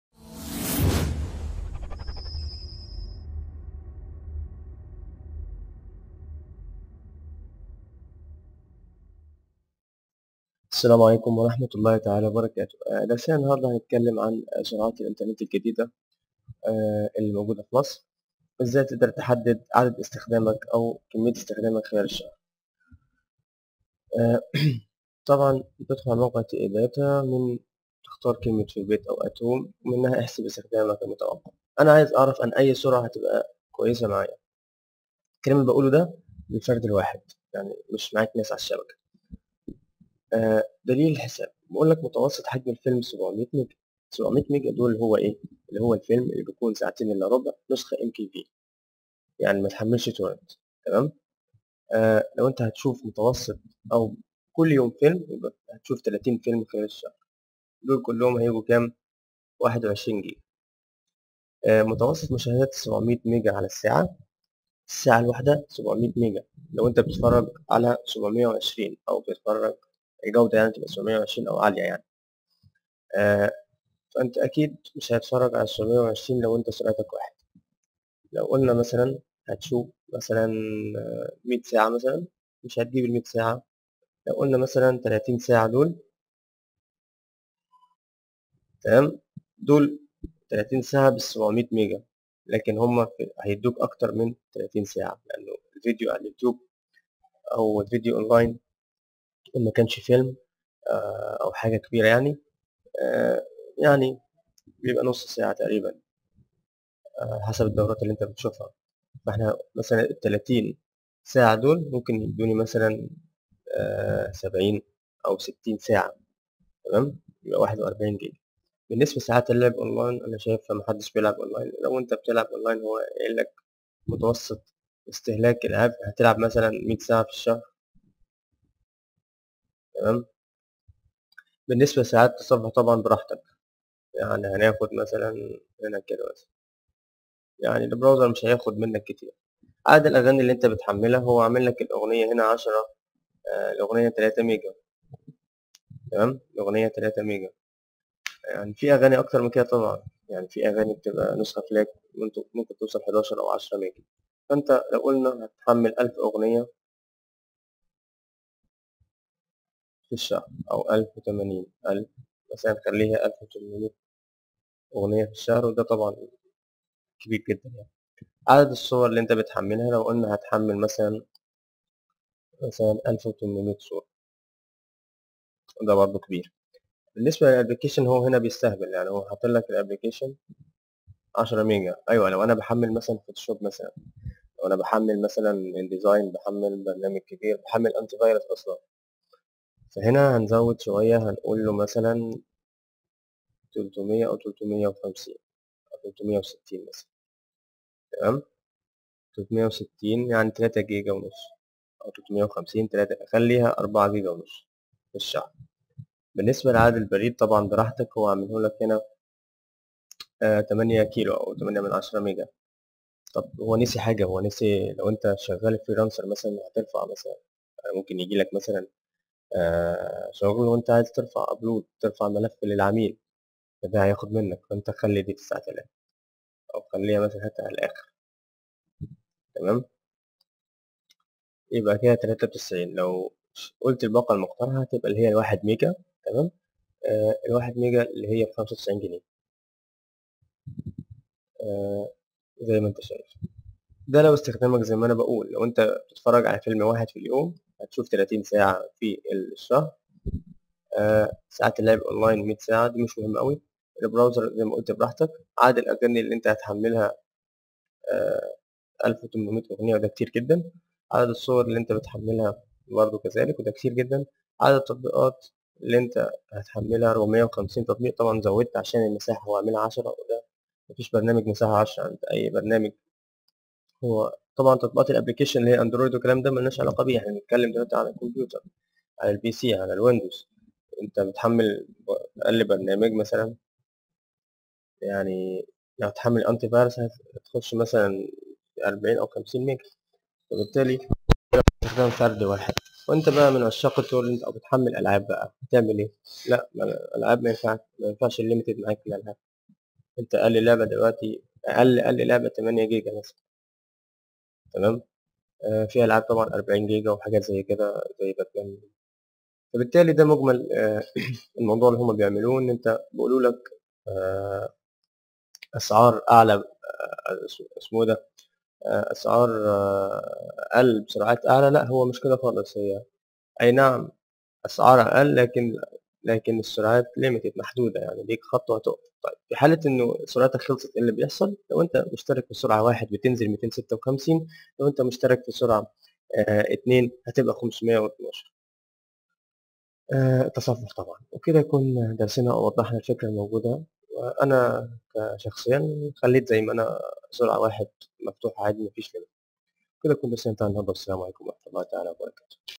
معرفة إستهلاك الإنترنت شهريا ومتوسط إستهلاك عدد الجيجات
كيف تـــقوم بمعرفة كم جيجا تستخدمها خلال ال 30 يوم . وحساب المتوسط والاجمالى عبر موقع تي اي داتا. ومن خلال ذلك تستطيع معرفة اي سرعة مناسبة لك وبطريقة سهله وبدون برامج.

لحل المشاكل والاستفسارات  عليك الانضمام الى صفحه الفيس بوك.